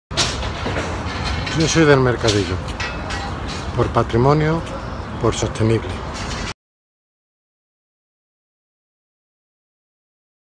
YoSoyDelMercadillo. Porque es sostenible
YoSoyDelMercadillo, una iniciativa de la Fundación Secretariado Gitano que apoya la venta ambulante tras la crisis del Covid-19.